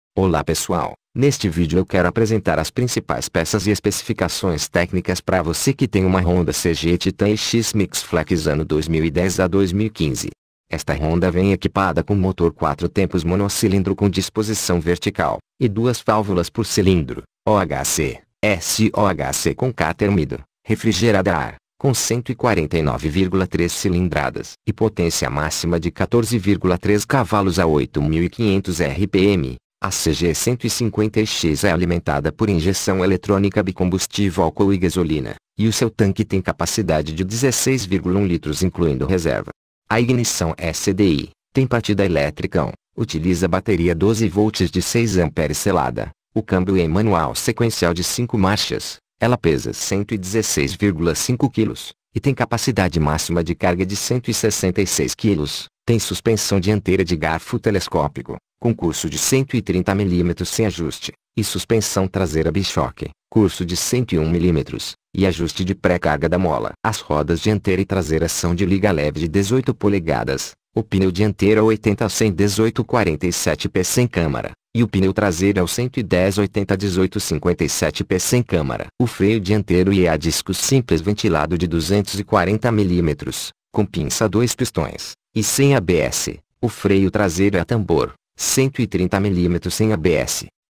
Ficha Técnica honda Cg 150 titan EX mix Flex ano 2010 2015
👍 COMPRE NO MAGAZINEVOLGRAN EM 10 X SEM JUROS  PARCEIRO MAGALU

Esta honda vem equipada com motor 4 tempos  monocilindro com  disposição vertical, e duas válvulas por cilindro, Ó H c, s ó h c  com cárter úmido, refrigerada a ár,  com 149,3 cilindradas. E potência máxima de 14,3 cavalos a 8500 RPM,

A cg 150 EX É alimentada por injeção eletrônica  bicombustível  álcool e gasolina , e o seu  tanque tem  capacidade de 16,1 litros incluindo reserva,

a Ignição é  CDI  ,tem partida a pedal, utiliza bateria 12 voltes de 6 âmpéres selada,
o Câmbio é manual sequencial de 5 marchas,
Ela peza  116,5 kilos, e tem capacidade máxima de carga de 166 kilos,
Têm suspensão dianteira de garfo telescópico , com curso de 130 mm  sem ajuste, e suspenção traseira  bichóque, curso de 101mm, e ajuste de pré carga da mola .
As rodas  dianteira e traseira são  de liga leve  de  18 polegadas,
O pneu dianteiro é o 80/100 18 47P sem câmara,  e o pneu traseiro é o 110/80 18 57P sem câmara.
o  freio dianteiro e á disco simples ventilado de 240mm, com pinça 2 pistões, e sem a b s , o freio traseiro é  a tambor ,130mm sem a b s,